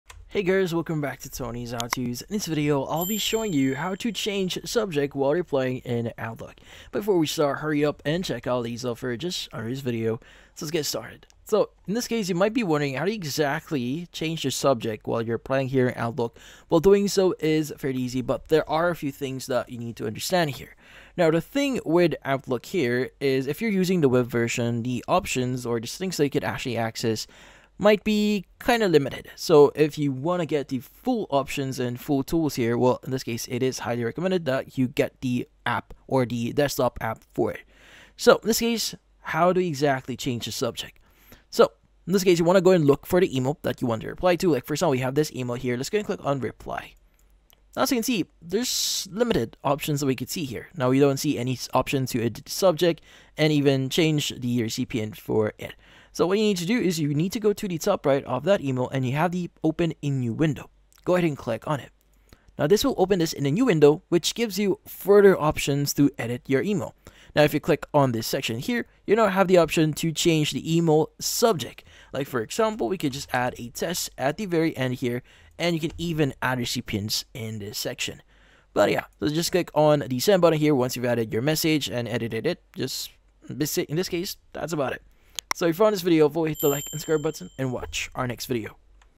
How to Change Subject While Replying in Outlook (2026)
In this video, I show you How to Change Subject While Replying in Outlook in 2026. The quick and easy tutorial Can I edit subject line in outlook when replying. Do you want to know step by step how to insert custom subjest in otlook reply in 2026? Yes? Great!